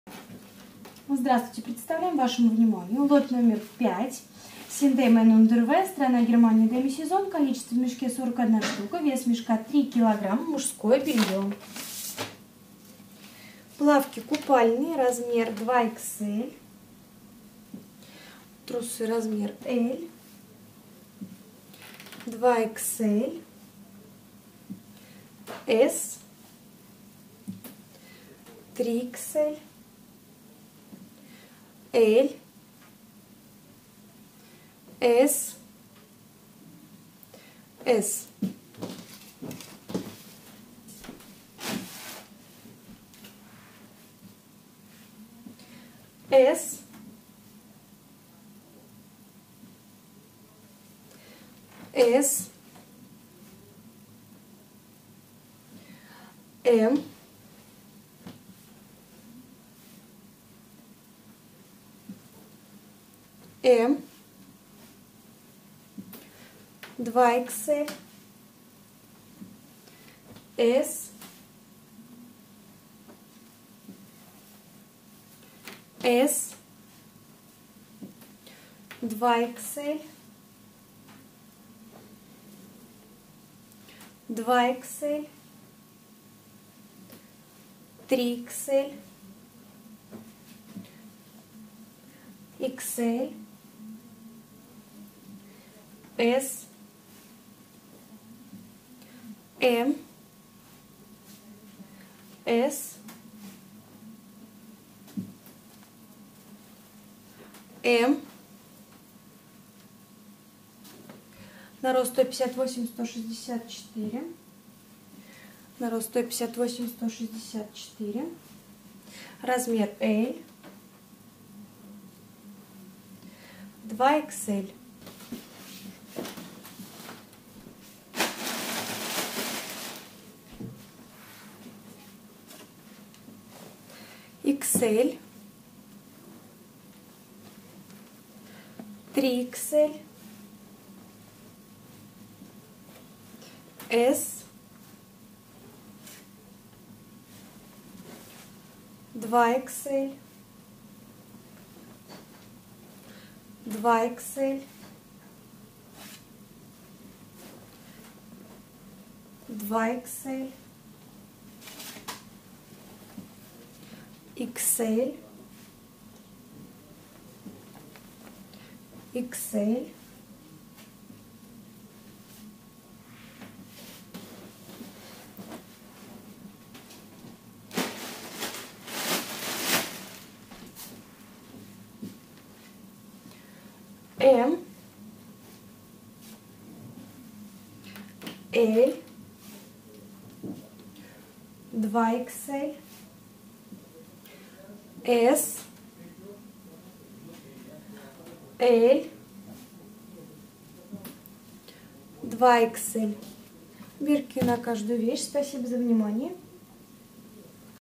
C&A MEN UNDERWEAR 5 ,Цена за кг : 17.9 евро ,сток одежда оптом
C&A MEN UNDERWEAR ( лот № 5 )
Мужское бельё ( сток )
Страна: Германия
Сезон: Демисезон
Цена за кг.: 17.9  евро
Цена за мешок: 53.7 евро
Цена за ед.: 1.31 евро